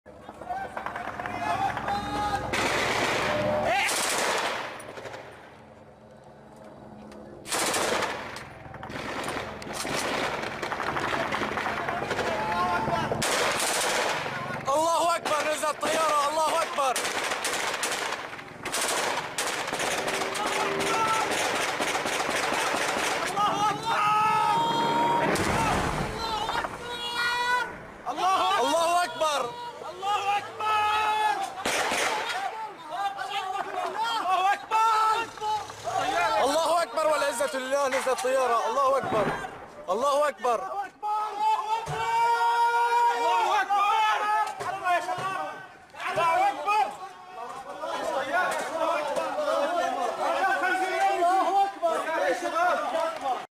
"الحر" يسقط مروحية في ريف إدلب
أفاد ناشطون سوريون أن الجيش الحر أسقط طائرة مروحية تابعة للجيش السوري في بلدة بسيدا جنوبي معرة النعمان في ريف إدلب. لمشاهدة المزيد يرجى زيارة الموقع الإلكتروني لسكاي نيوز عربية من خلال الضغط على الرابط التالي: